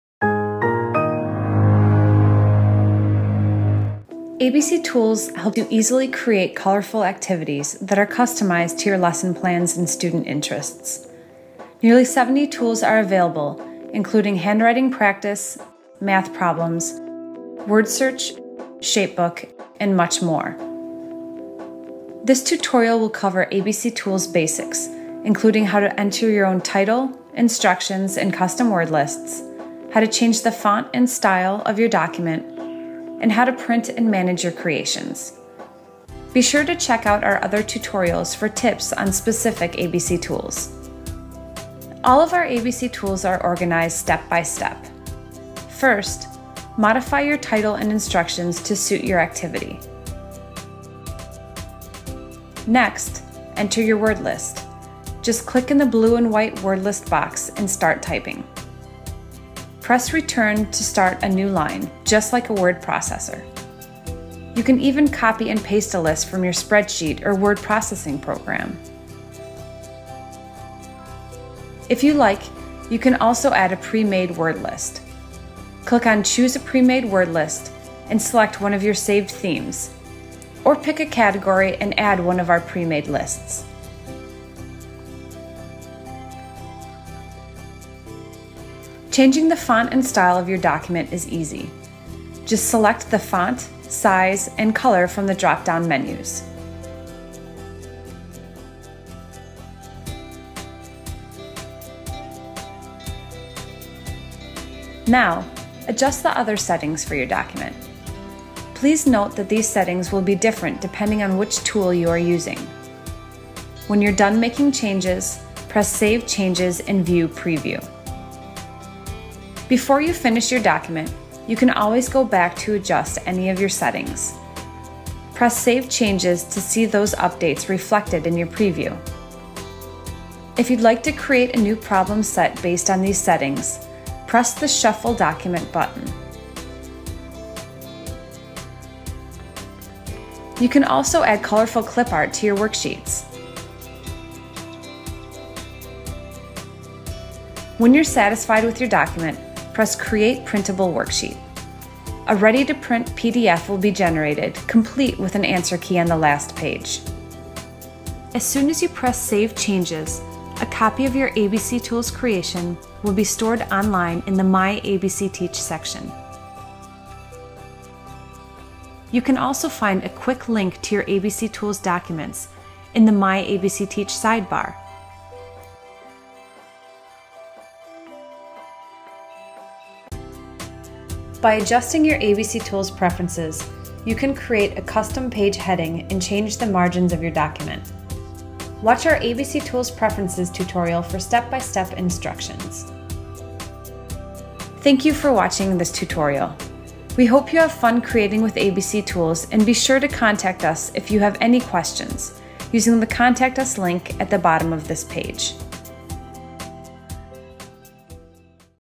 Create your own custom games and activities with abctools (abcteach.com)
This tutorial describes how to use the advanced abctools available to abcteach members.